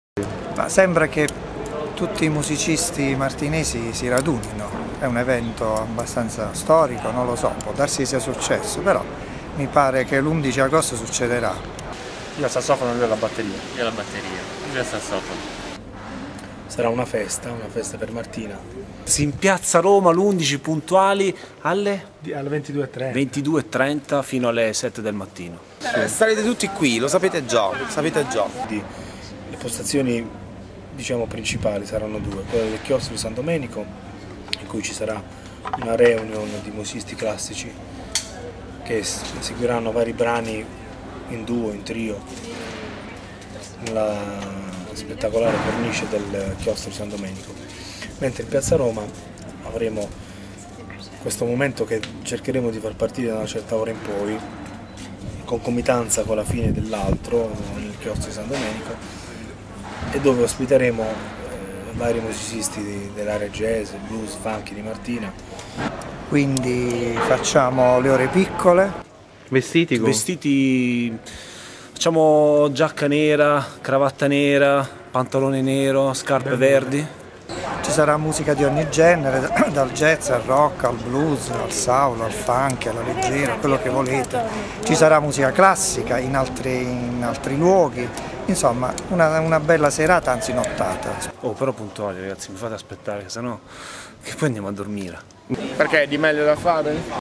That's Martina - La notte. Tanto che avete di meglio da fare?
Spot degli artisti di Martina Franca che invitano i cittadini a partecipare agli eventi di sabato 11 agosto 2012, organizzati dal Comune di Martina Franca insieme a tantissime associazioni della città.